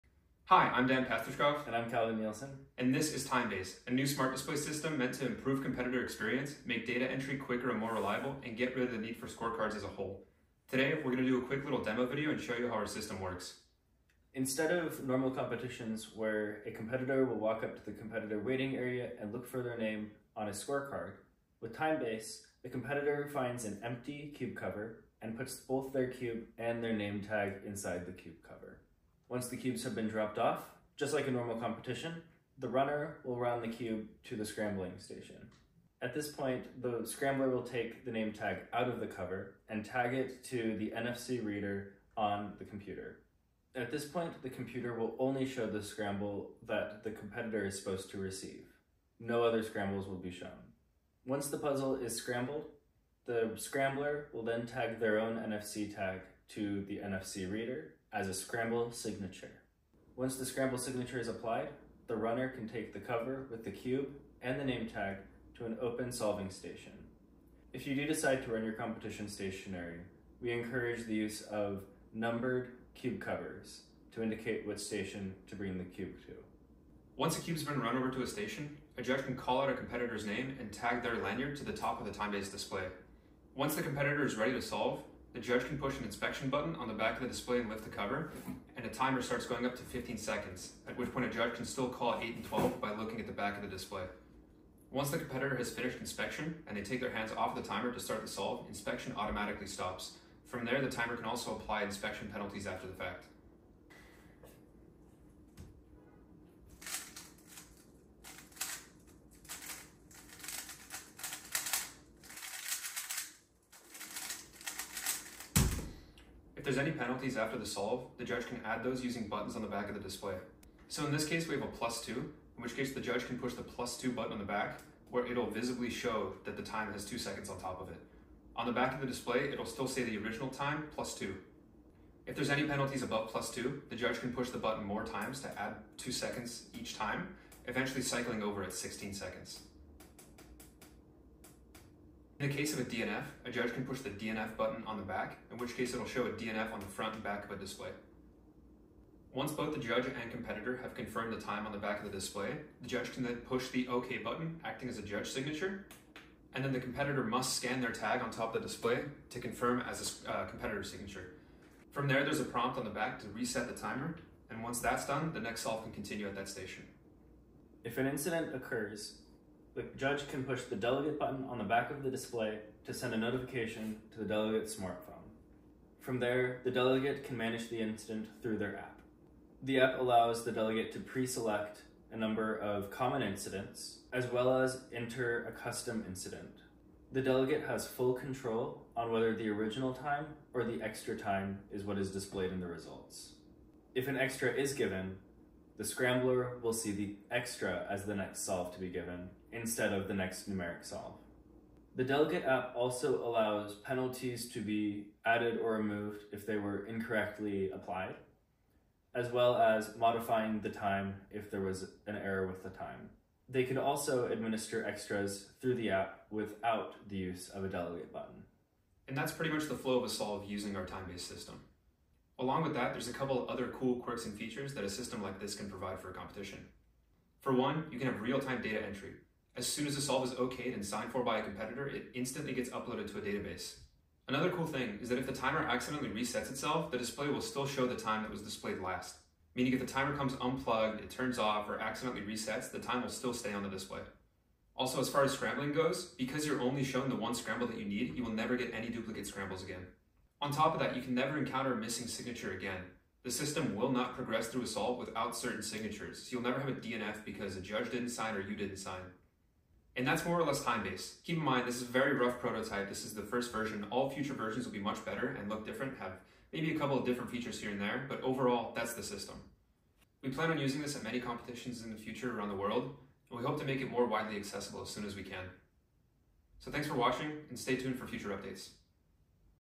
TimeBase Prototype Showcase
In this video we go over the basic procedure of our TimeBase system. This is the first prototype so expect differences in later versions.

The system was first showcased in CubingUSA Nationals 2023 and sparked greater interest into how the system works. This video showcases what we debuted during our seminar in greater detail.